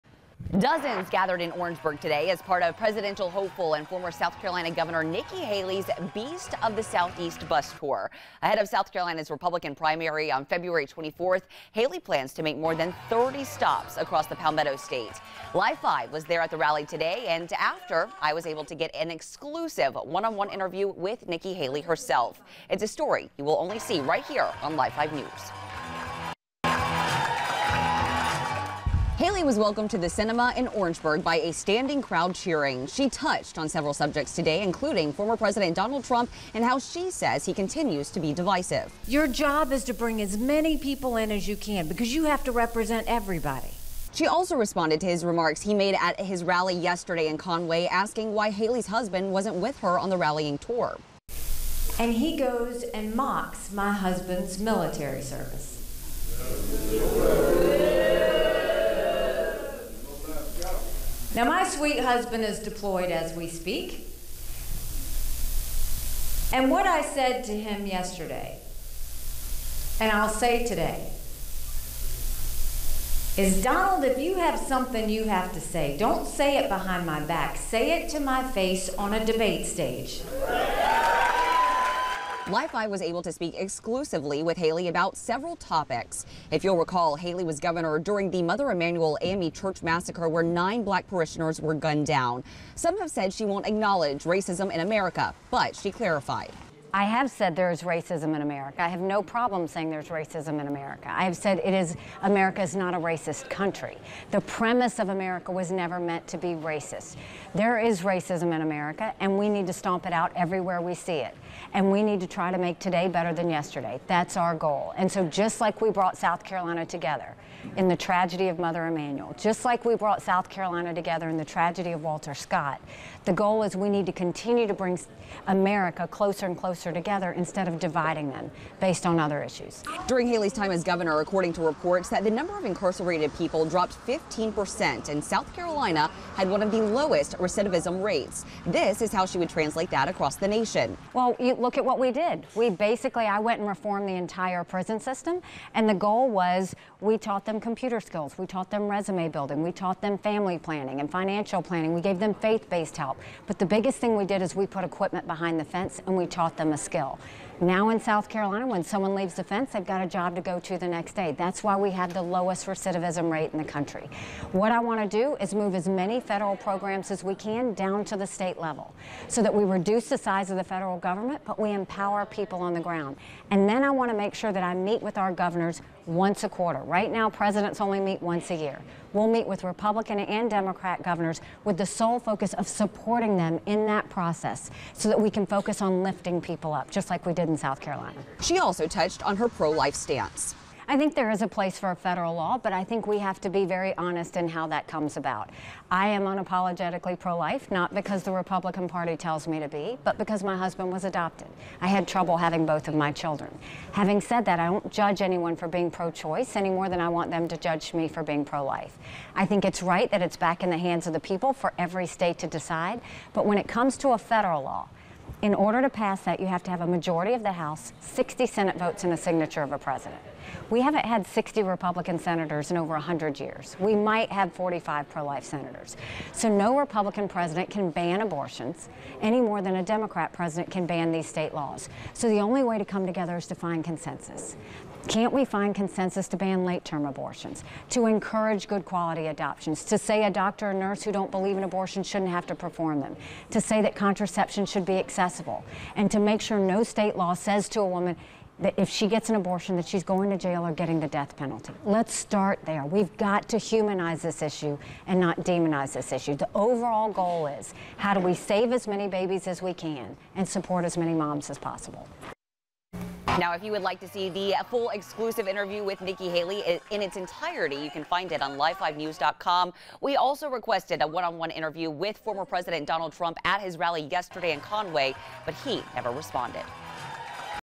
VIDEO: Haley talks about changes she would make as president in exclusive interview
With a little under two weeks to go before the South Carolina Republican primary, GOP Presidential hopeful Nikki Haley made another stop in her home state this